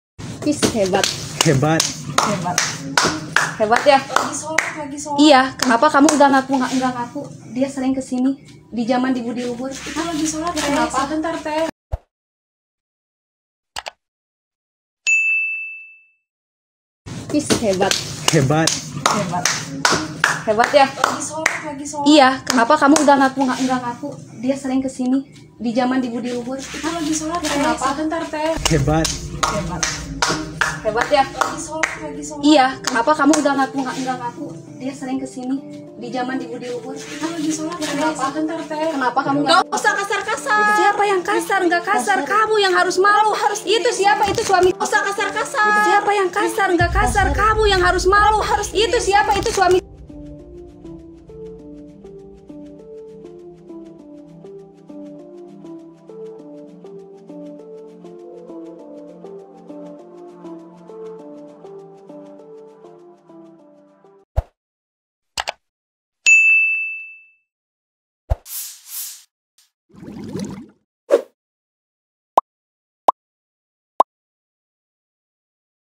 Pria Ini Pura-pura Salat saat Ketahuan Selingkuh dengan Pelakor
Sebuah video memperlihatkan istri sah melabrak suaminya yang tengah berduaan dengan wanita lain.

Mereka digerebek istri sah dan keluarga di kosan wanita selingkuhannya.

Saat digerebek, wanita tersebut tampak terkejut dan bangun dari tempat tidurnya.

Sang suami tiba-tiba pura-pura salat saat istrinya datang.

Video terkini dan terbaru..